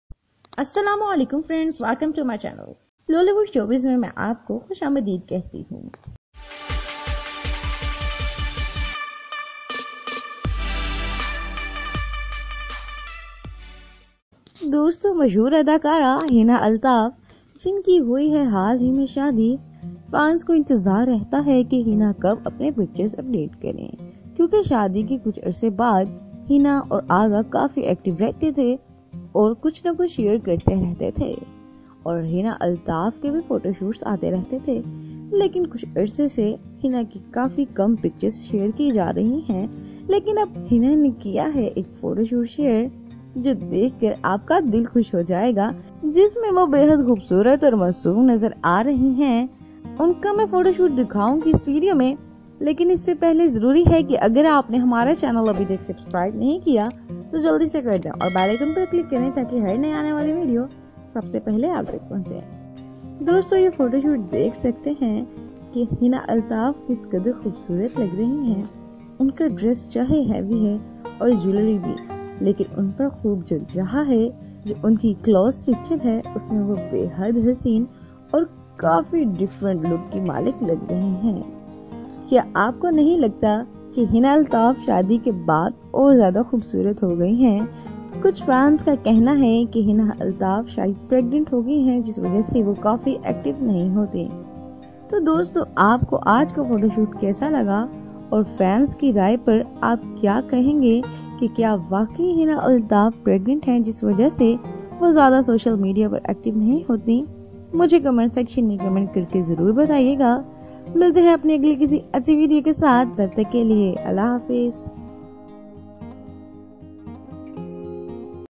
Hina Altaf and Aagha Ali Become Three Member Soon || Lollywood Showbiz
Hina altaf latest bridal shoot looking most beautiful
Hina Altaf and Aagha Ali Become Three Member Soon
Hina altaf and Aagha ali